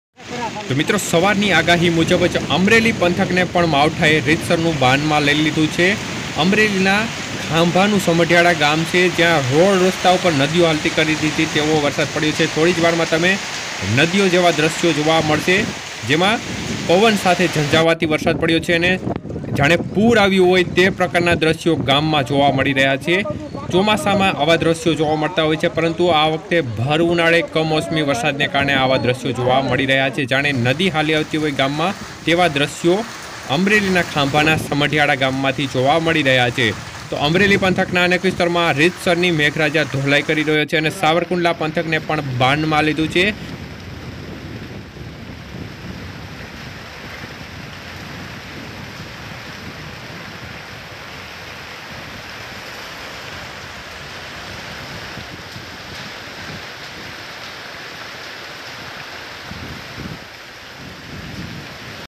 ખાંભા ના સમઢિયાળા મા ભર ઉનાળે રોડ પર નદીઓ, માવઠા નો વરસાદ તો જોવો
વરસાદ ના સમાચાર |વરસાદ ની આગાહી |weather